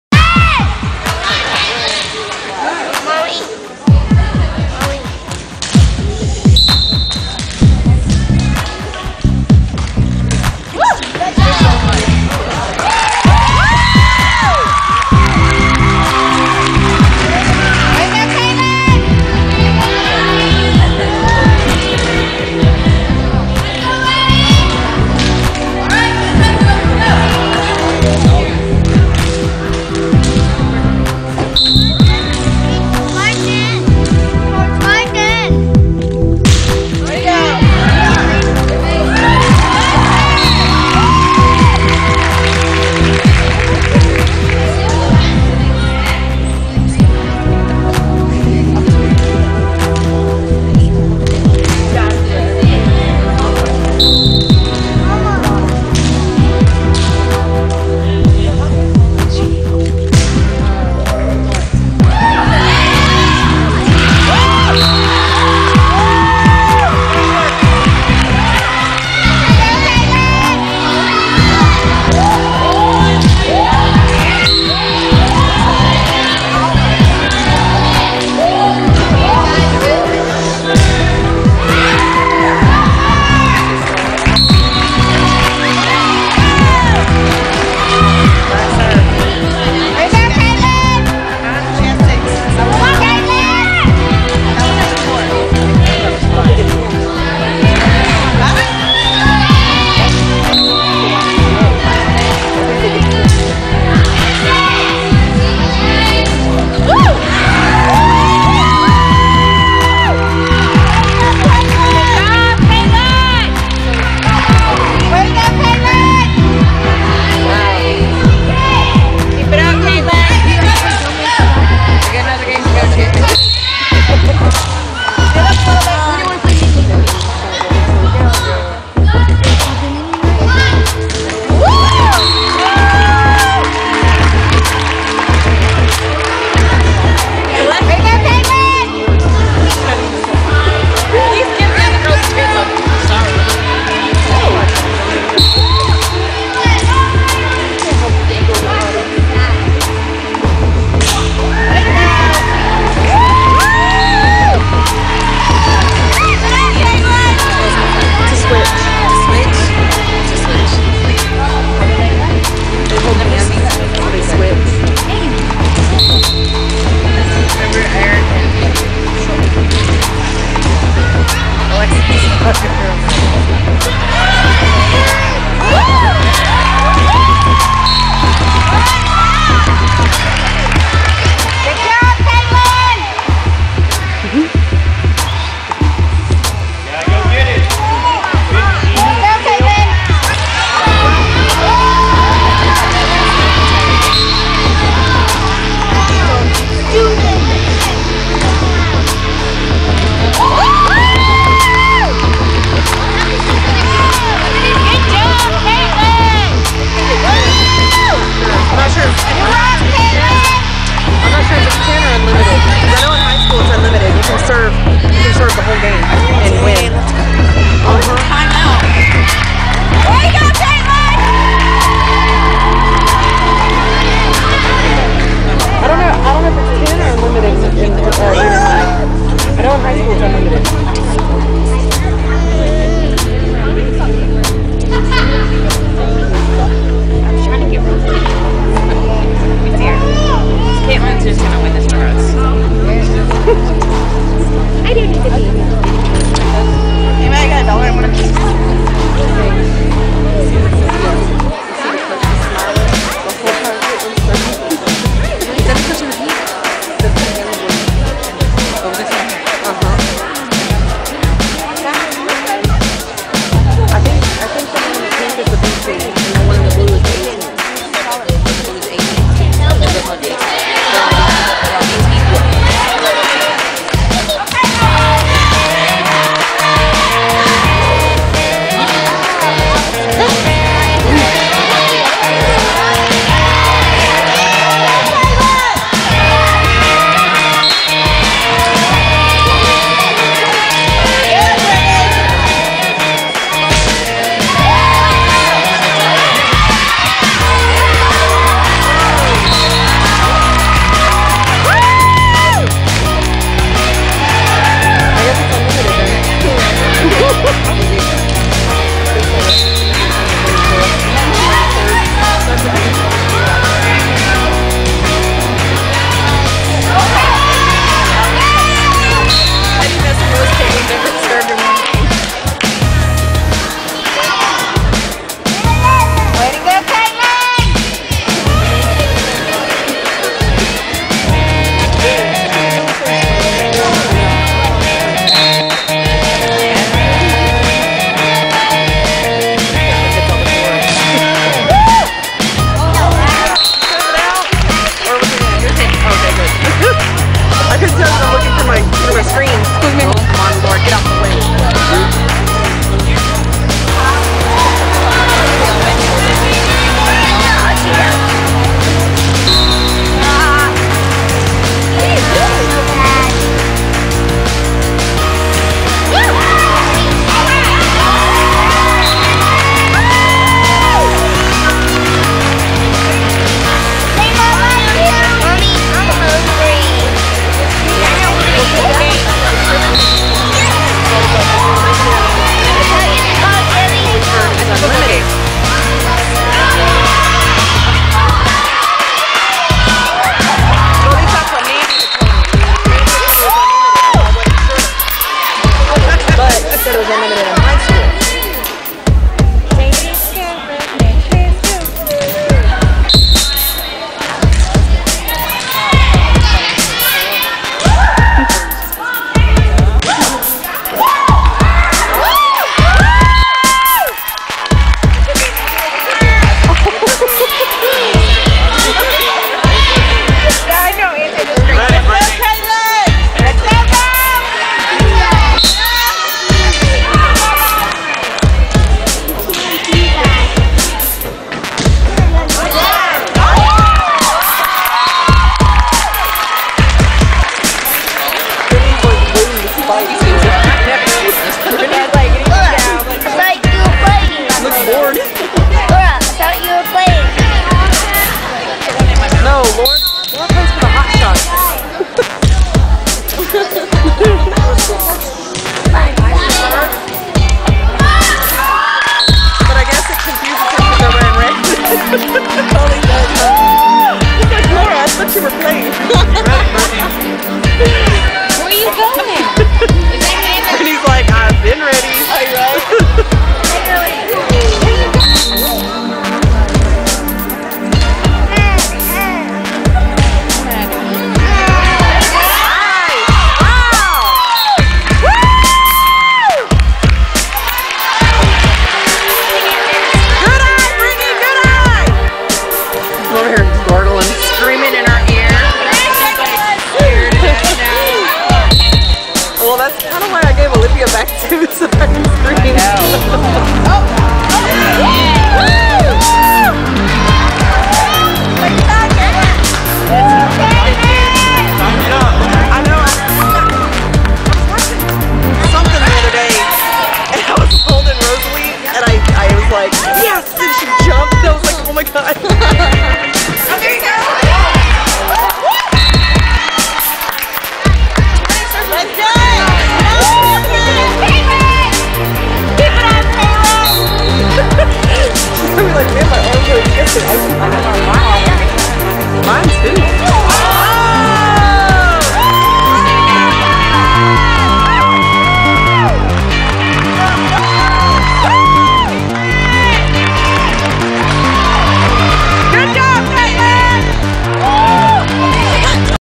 Kaitlyn gets 20 serves
Lady Roughnecks game 1 Sept. 12, 2013. Kaitlyn goes up as first server, match 1 and gets 20 points for East Roughnecks.